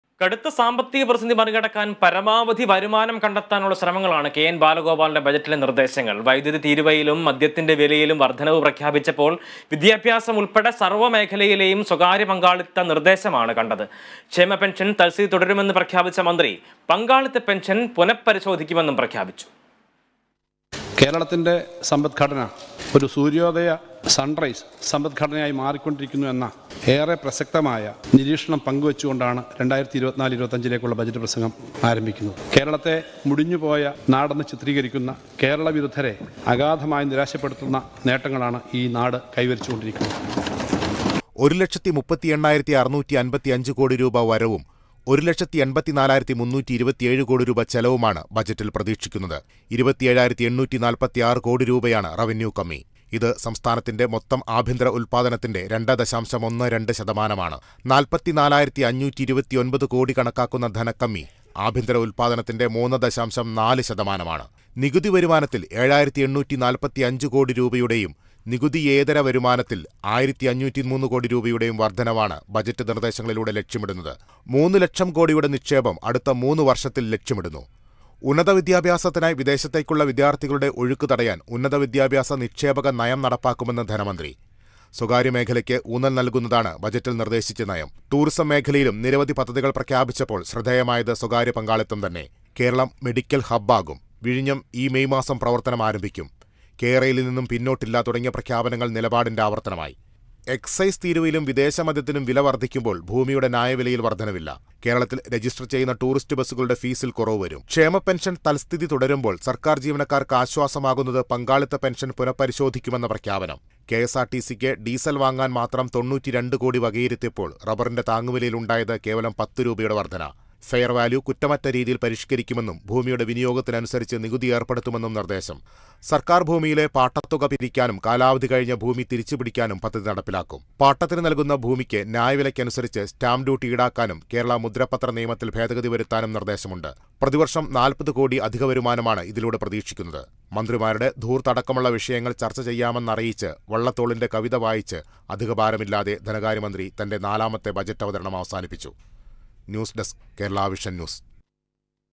ബജറ്റ് സാമ്പത്തിക പ്രതിസന്ധിയില്‍ നിന്നും കരകയറാനുള്ള കച്ചിതുരുമ്പോ? | Kerala Budget
കടുത്ത സാമ്പത്തിക പ്രതിസന്ധി മറികടക്കാന്‍ പരമാവധി വരുമാനം കണ്ടെത്താനുള്ള ശ്രമങ്ങളാണ് കെ എന്‍ ബാലഗോപാലിന്റെ ബജറ്റിലെ നിര്‍ദ്ദേശങ്ങള്‍. വൈദ്യുതി തീരുവയിലും മദ്യത്തിന്റെ വിലയിലും വര്‍ദ്ധനവ് പ്രഖ്യാപിച്ചപ്പോള്‍ വിദ്യാഭാസമുള്‍പ്പെടെ സര്‍വ്വ മേഖലകളിലെയും സ്വകാര്യ പങ്കാളിത്ത നിര്‍ദ്ദേശമാണ്. ക്ഷേമ പെന്‍ഷന്‍ തല്‍സ്ഥിതി തുടരുമെന്ന് പ്രഖ്യാപിച്ച മന്ത്രി പങ്കാളിത്തപെന്‍ഷന്‍ പുന പരിശോധിക്കുമെന്നും പ്രഖ്യാപിച്ചു.

About COA